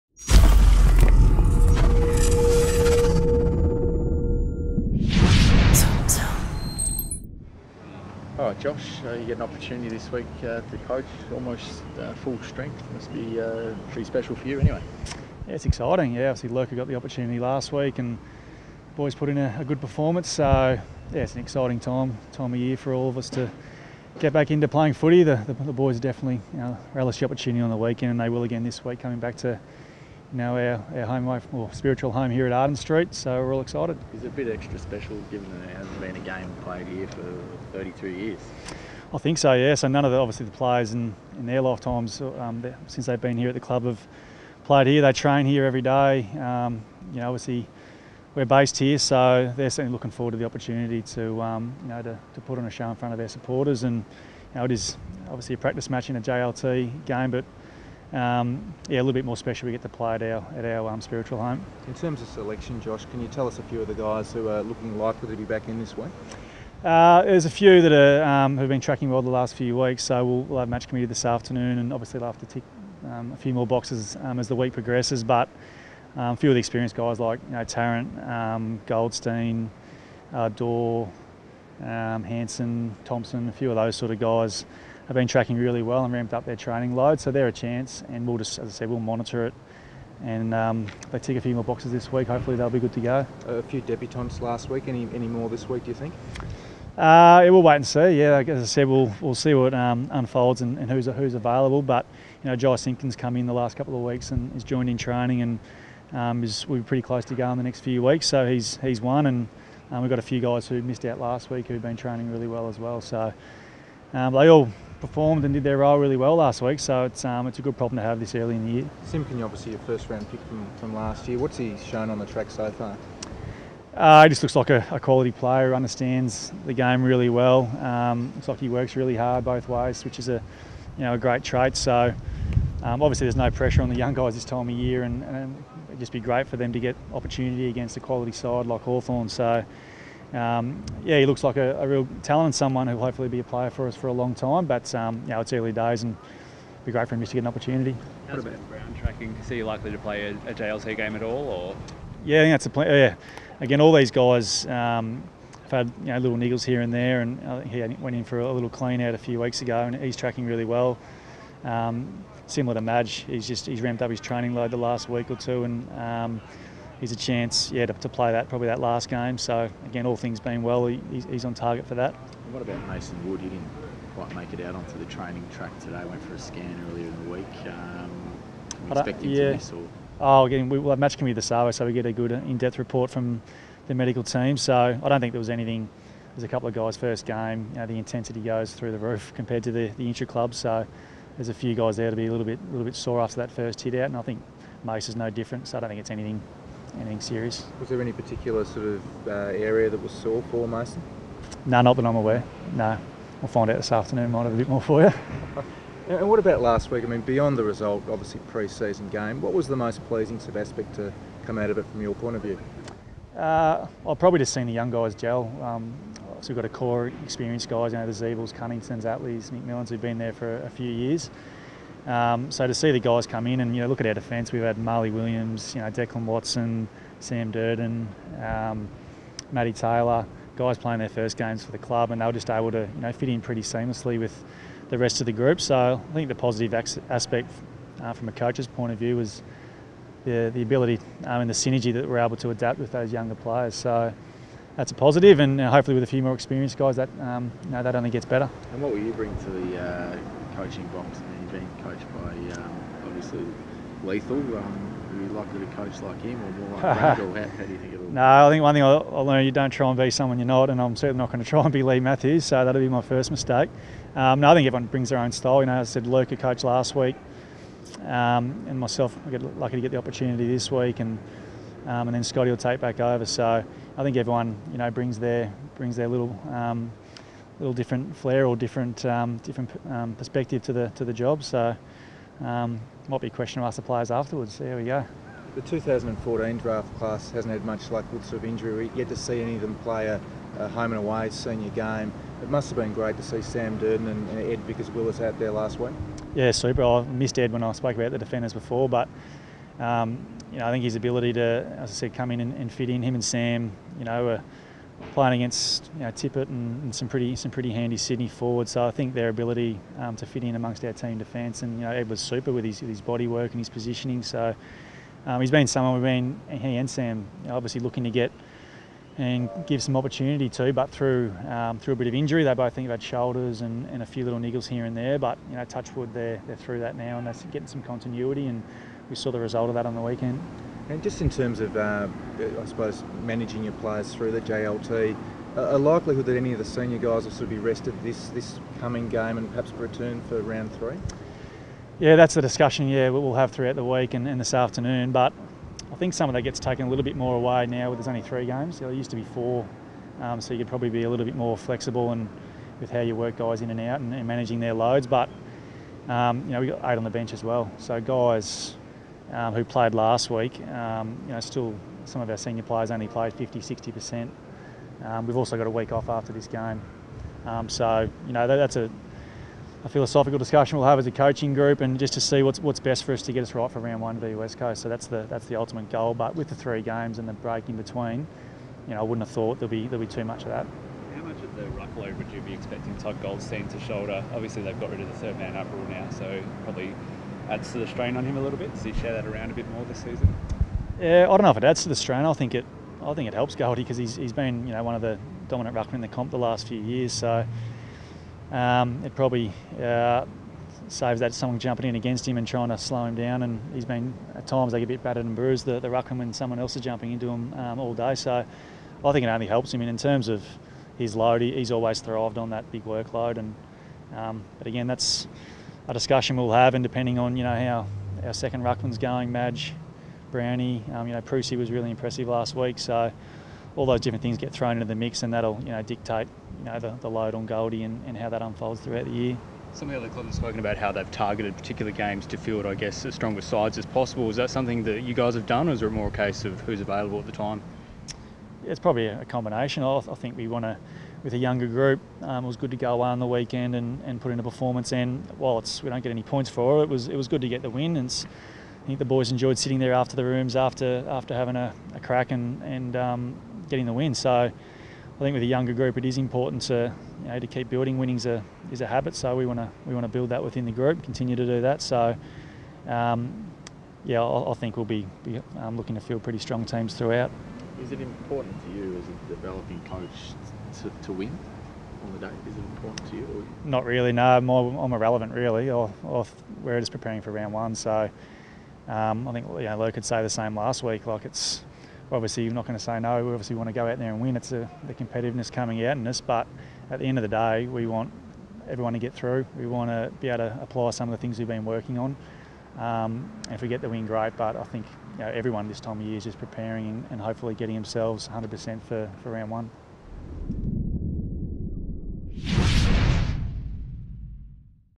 February 22, 2017 - Josh Drummond media conference
Josh Drummond fronts the media ahead of the JLT Community Series match at Arden Street on Sunday.